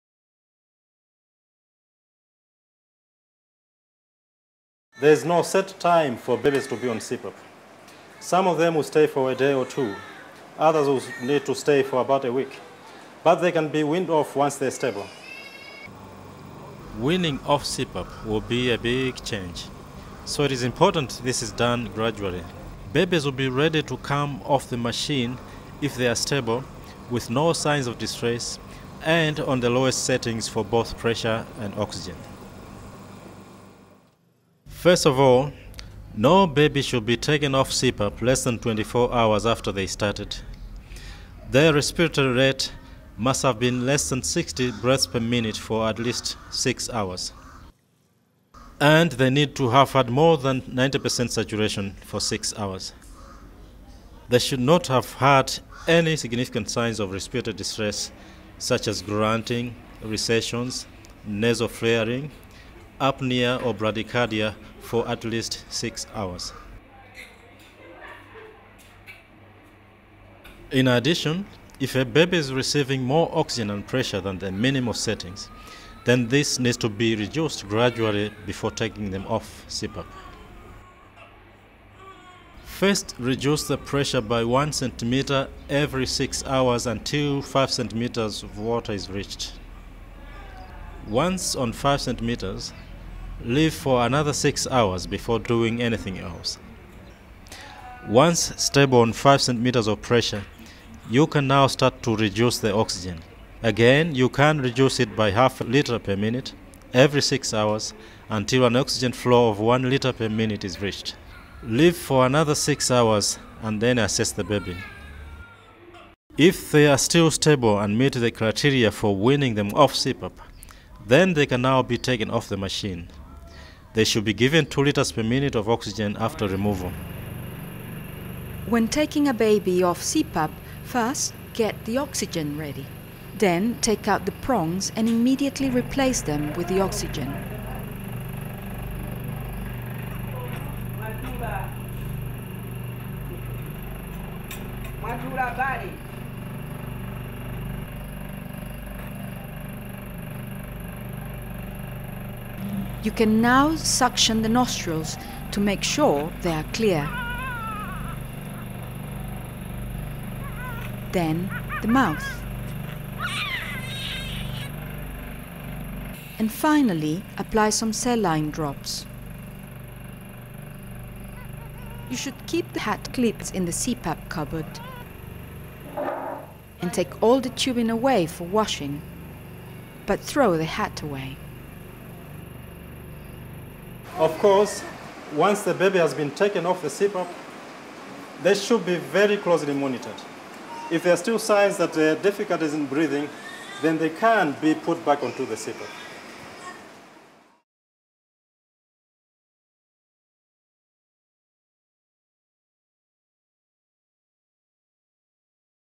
Weaning a baby off CPAP (Pumani bCPAP training films: Section 9 of 13).
This film shows how to assess a baby for weaning off CPAP and how to take them off the machine.

This is one of a set of 13 Pumani bCPAP training films made by partner organisation Picturing Health.

Originally developed by students from Rice University, Texas, the low cost Pumani Bubble CPAP is now being rolled out across Africa, and is becoming a basic piece of equipment in many hospitals and health centres. The CPAP project is run by Rice 360˚ Institute for Global Health Technologies and Hadleigh Health Technologies, with funding from USAID. Permission must be obtained for other uses.